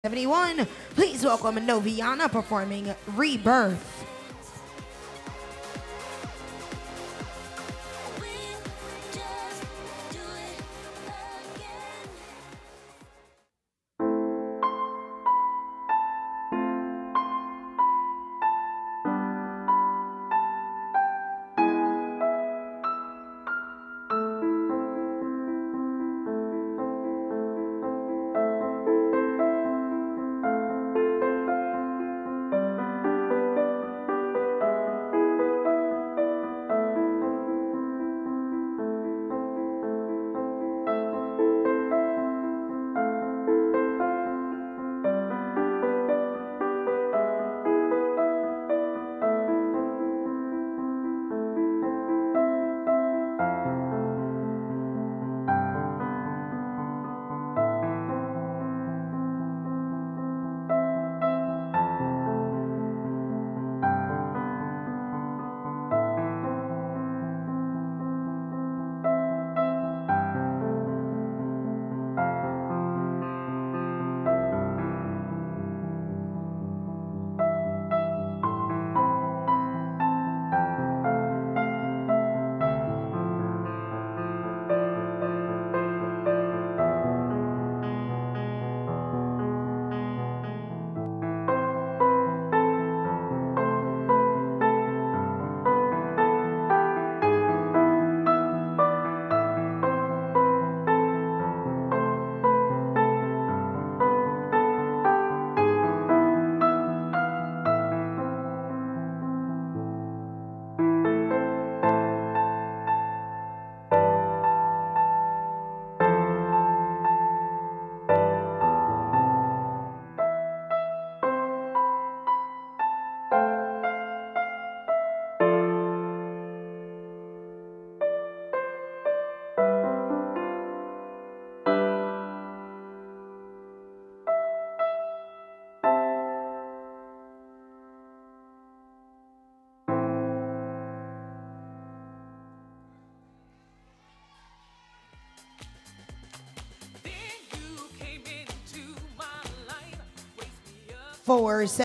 Rebirth | Noviana Lin
"Rebirth" Contemporary Solo

CHOREOGRAPHY:
choreographed by Tommy Tibball
TKO dance academy

PLACEMENTS:
breakout dance competition
1st overall age 9 advanced soloist
title: petite miss breakout 2022

hall of fame dance challenge 2022
grand slam age 9 advanced soloist

starpower talent competition 2022
3rd overall 5 star elite age 9 advanced soloist
2nd runner up junior miss starpower